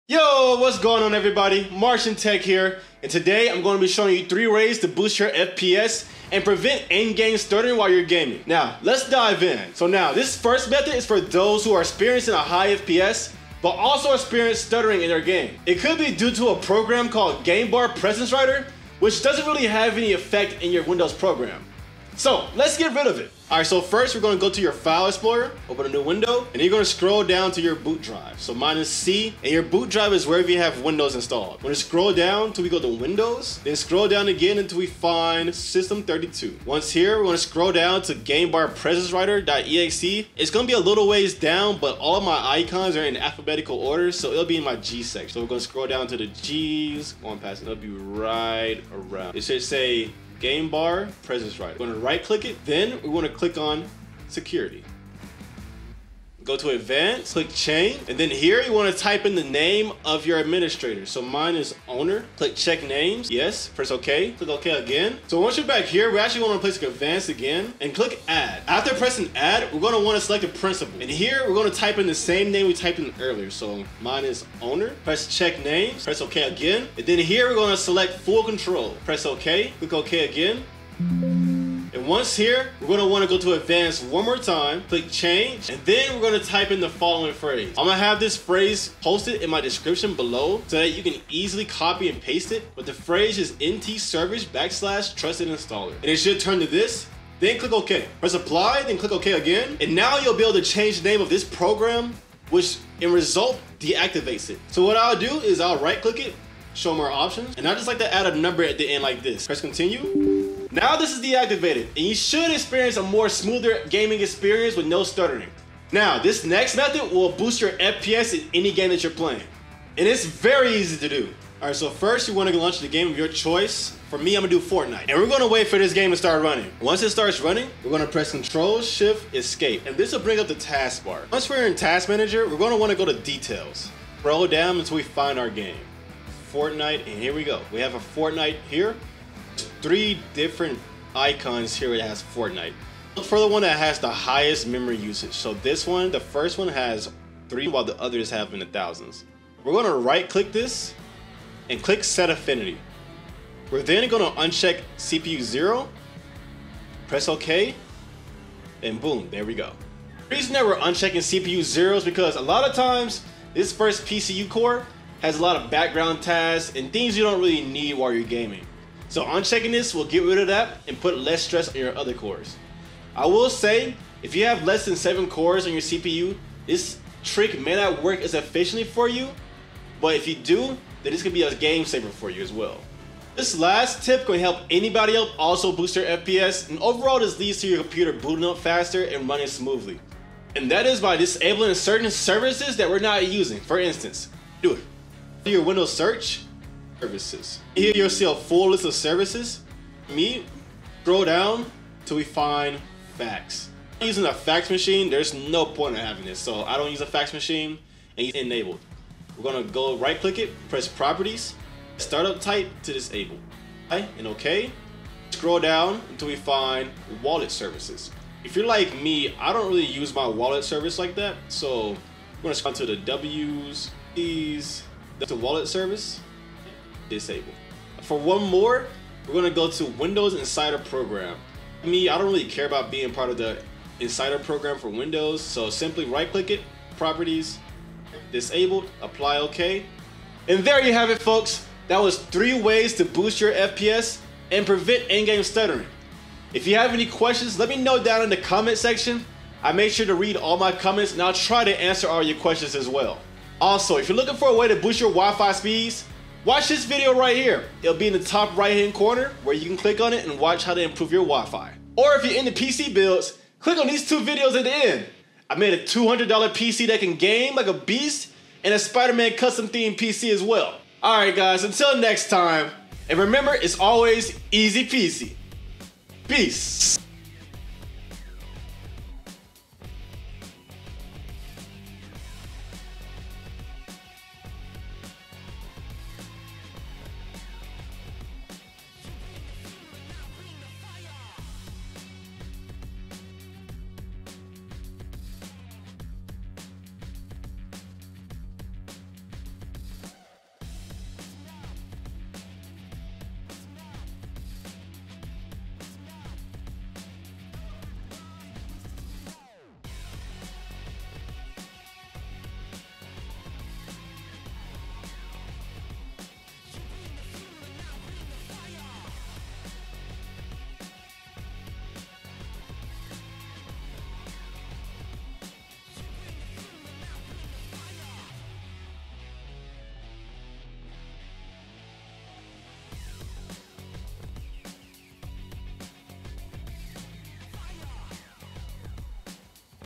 Maximize Your FPS: 3 Game-Changing Tips! 🔥
In today's video, we're diving deep into three game-changing methods to elevate your FPS and combat game stuttering! Whether you're a pro-gamer or just someone looking for smoother gameplay, these tips are for you. Let's supercharge your gaming experience together! 🔥🎮

🚀 Chapters:
0:00 - Introduction
0:25 - Method 1: Disable GameBarPresenceWriter.exe
2:02 - Method 2: Set Affinity in Task Manager
3:16 - Method 3: Disable Unneeded Services
4:20 - Outro

💬 Got questions or feedback? Your input helps shape our future content.

Boost FPS 2023
Fix Game Stuttering
Optimize PC Gaming